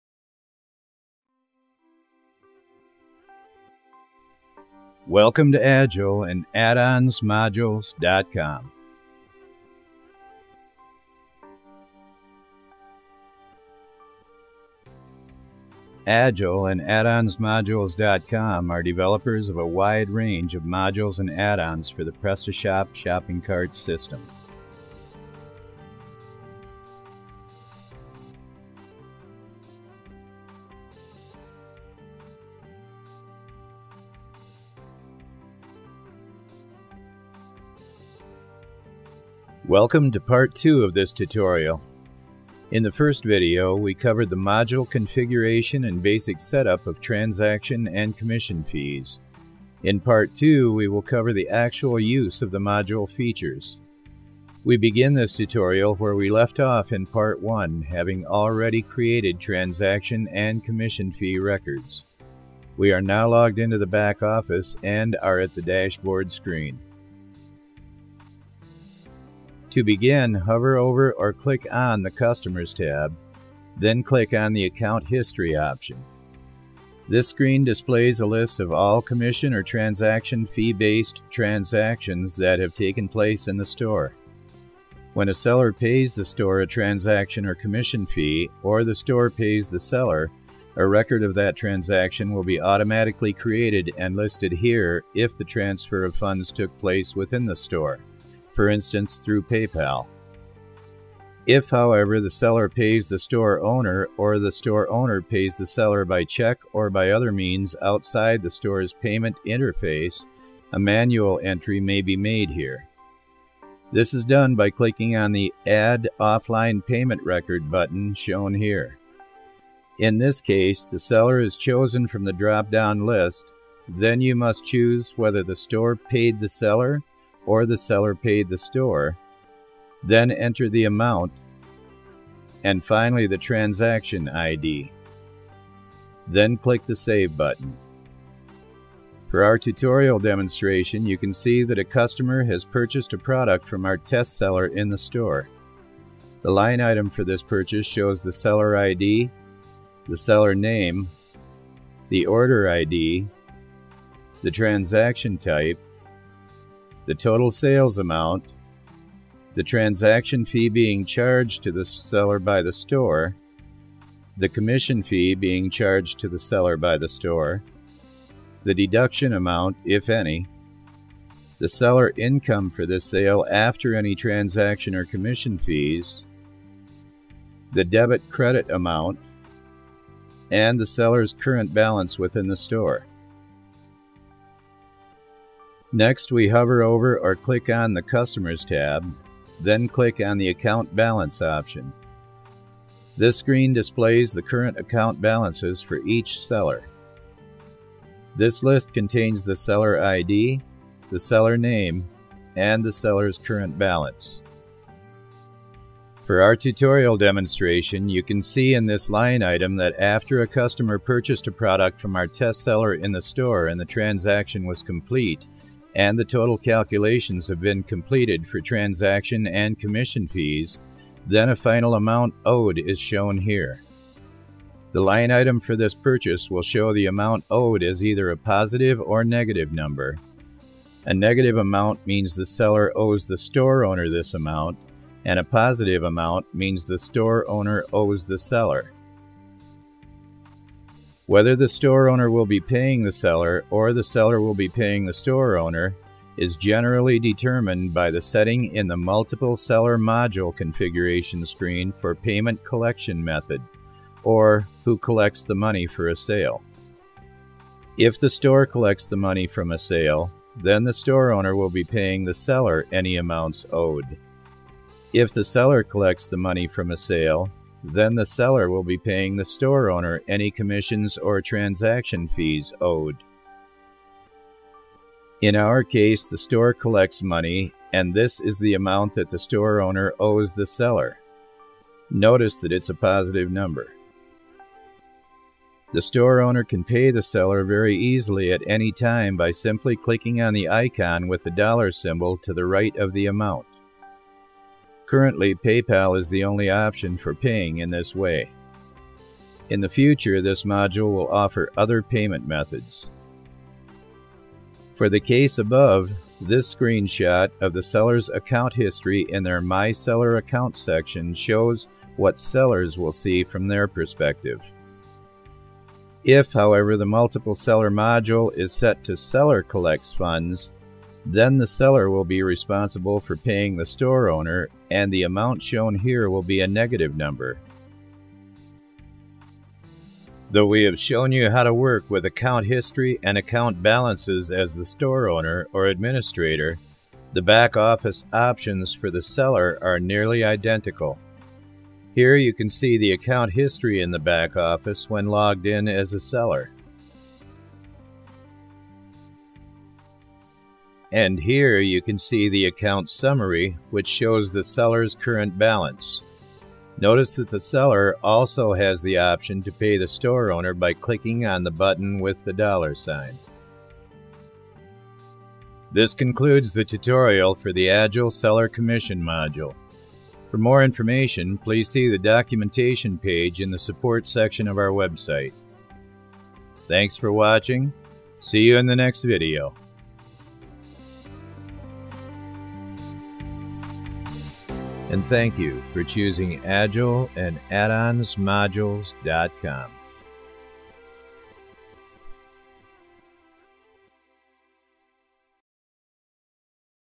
Agile Seller Commission Tutorial - Part Two
This video is part two of a tutorial for the configuration and use of the Agile Seller Commission module for the PrestaShop 1.5x shopping cart system.

In this video, the store administrator can see the process for setting up the module, as well as features this module will add to the Agile Multiple Seller module and the use of those features.

Be sure to watch part one of the tutorial before viewing this video.

Also, please see our tutorials for the installation and configuration of the Agile Multiple Seller module.

Agile and Addons-Modules.Com is a top developer of PrestaShop based online marketplace solutions.